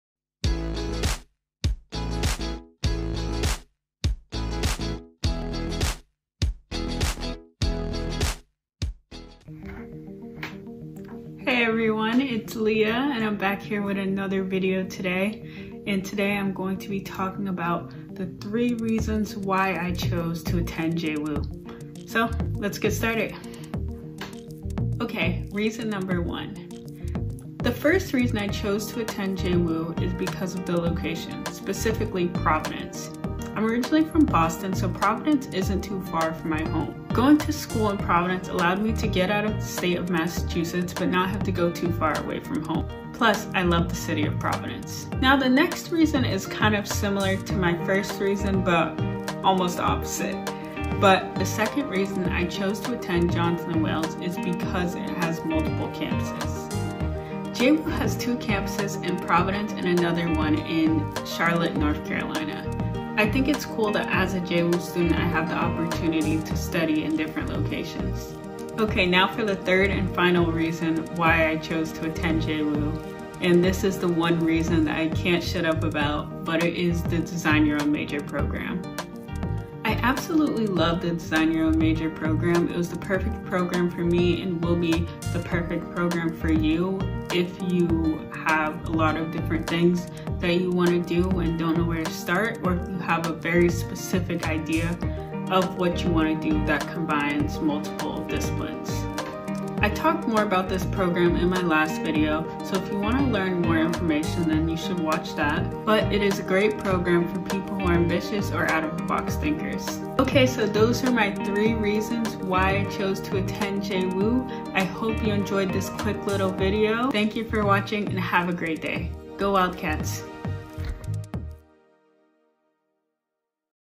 JWU Wildcat Vlog: Three Reasons Why I Chose Johnson & Wales University
Leah breaks down the three main reasons she chose Johnson & Wales University in Providence, RI! Before graduating high school, many students know where they want to attend college. For her, finding the perfect school was more of a journey. She loves JWU now and wouldn't want to be anywhere else, but it wasn't always her first choice. Here's her story

Keep up to date with everything JWU by following us on social!